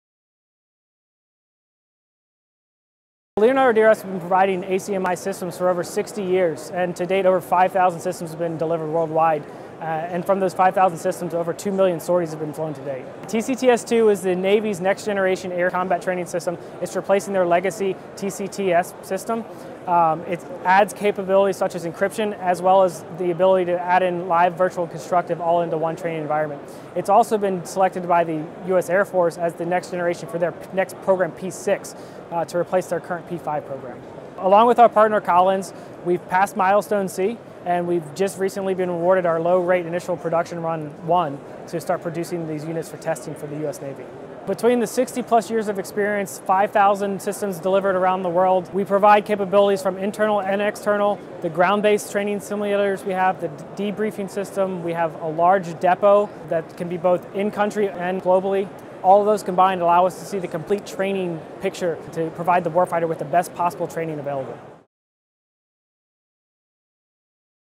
Air Combat Training
Since 1957 the Air Combat Training Systems (ACTS) product line of Leonardo DRS has developed and fielded high-performing systems that train the most lethal combat ready forces in the world.